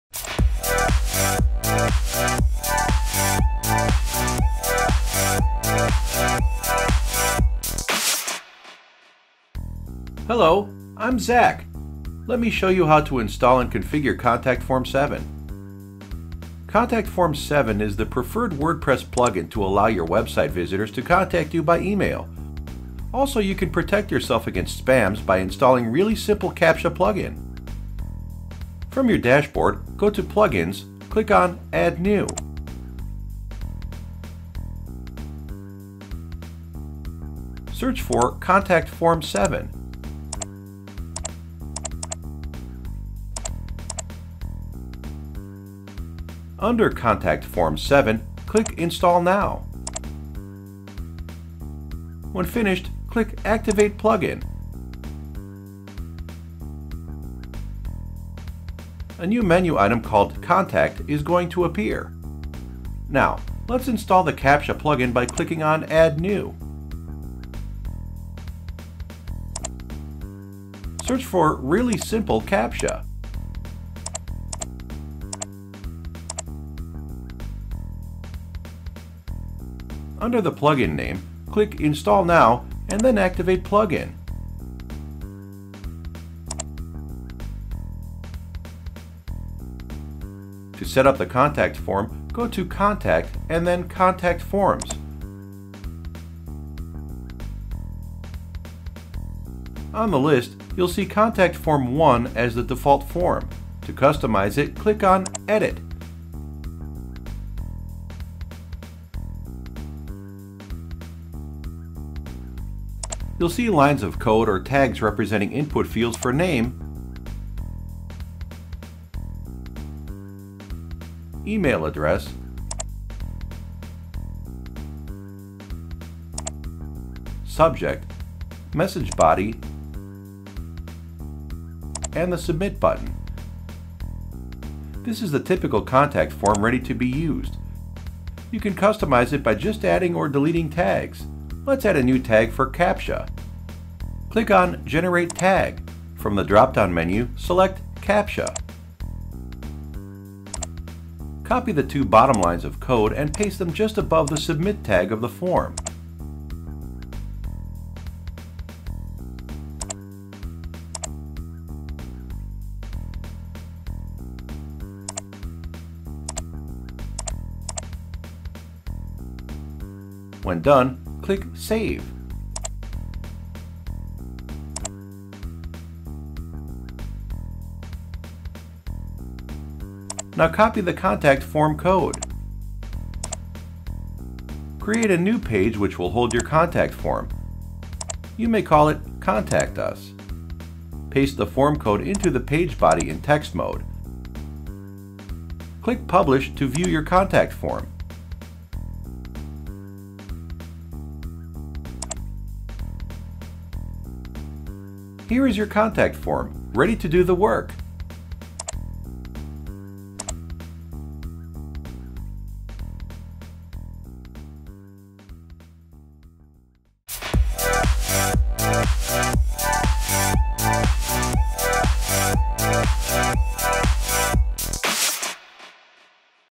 How to Install and Configure Contact Form 7 in WordPress
This video is about how to install and configure contact form 7.

Contact Form 7 can manage multiple contact forms, plus you can customize the form and the mail contents flexibly with simple markup.

0:30 - Go to plugin
0:48 - Install and activate contact form 7
1:04 - Add new plugin for captcha code
1:20 - Install and activate the plugin
1:30 - Set up contact form
2:20 - Add new tag
2:00 - Copy contact form code
3:10 - Create a new page for contact and paste the code